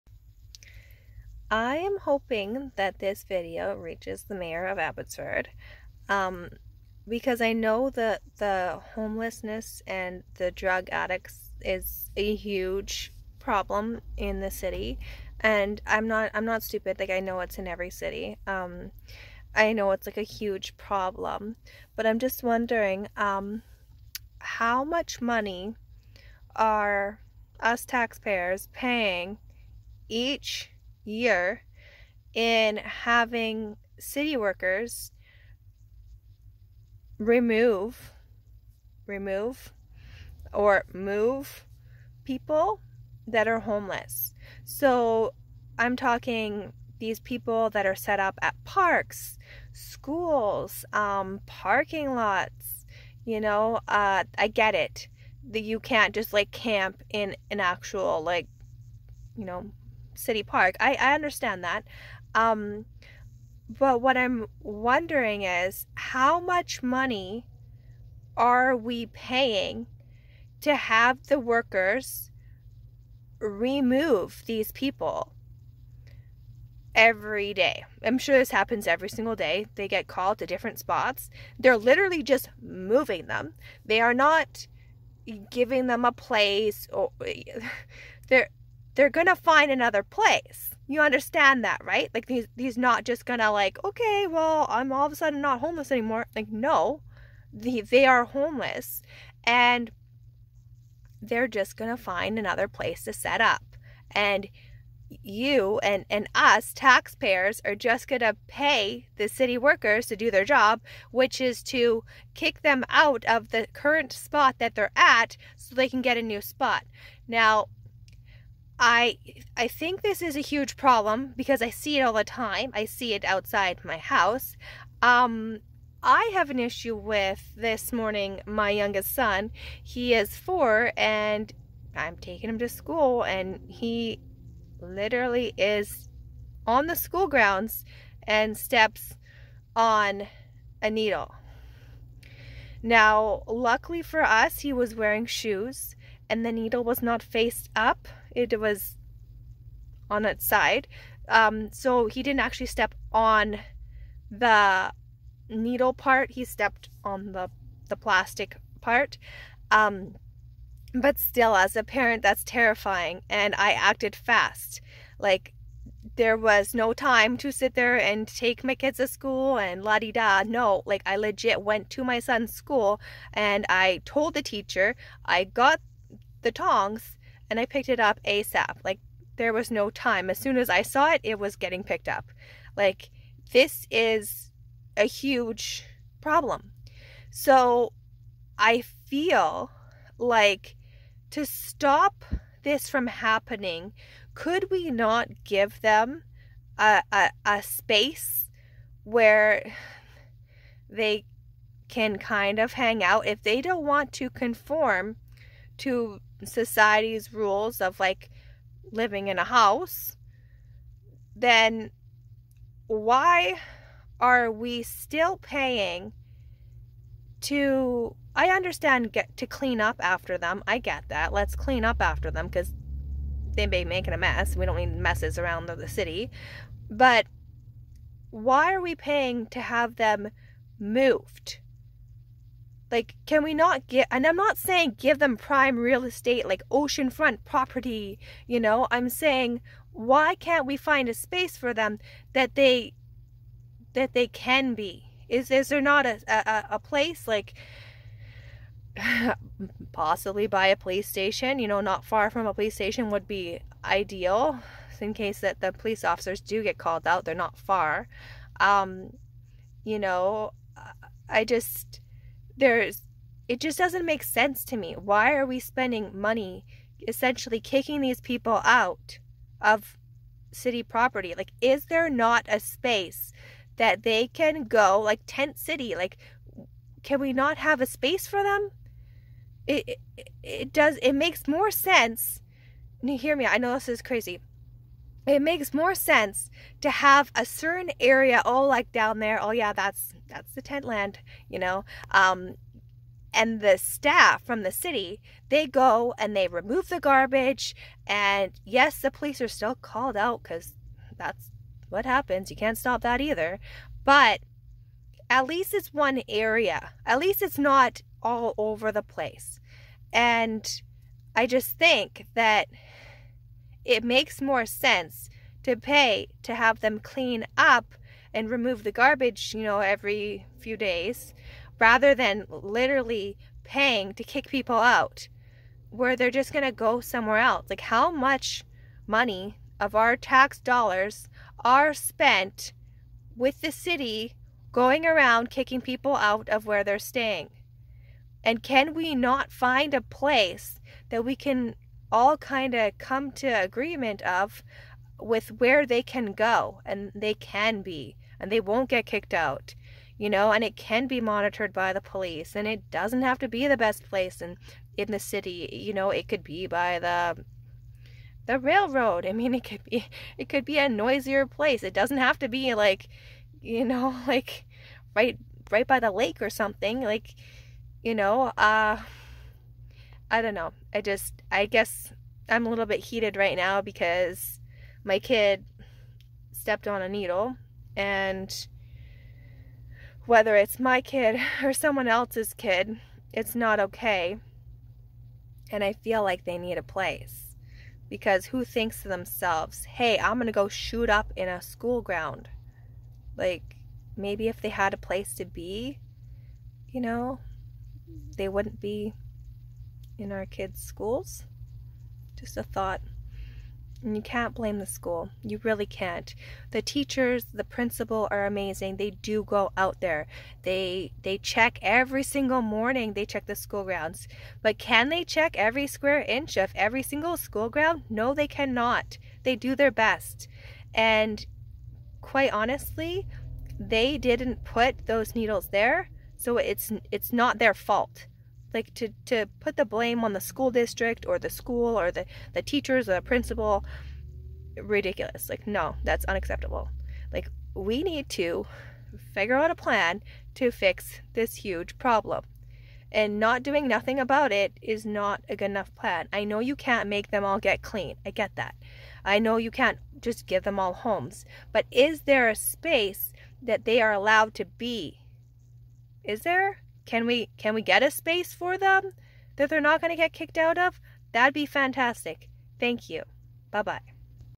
Mayor Henry Braun, Please watch!
This is a huge problem. I get that every city deals with it but what is Abbotsford doing to minimize these issues? The issues of drug addicts and homelessness. Give them an area so that they dont take over our parks, schools, and parking lots.